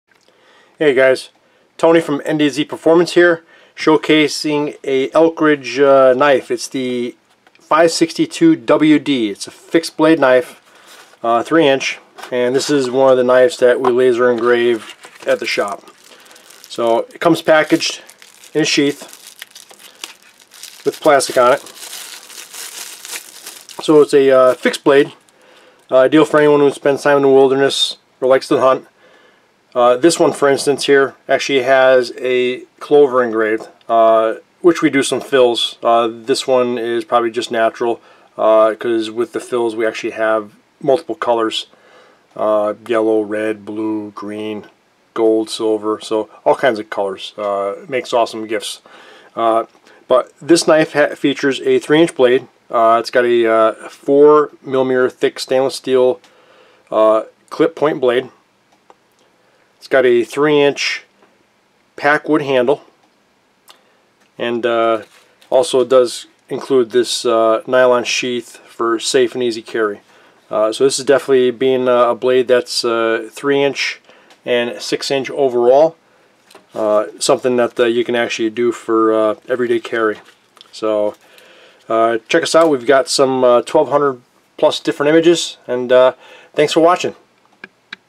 Elk Ridge ER-562WD Fixed Blade Knife engraved knife by NDZ - gift ideas
Here we have a quick look at one of the knives NDZ Performance laser engraves. The Elk Ridge ER-562-WD has a 6" overall length, with a 3" blade. Includes a nylon sheath.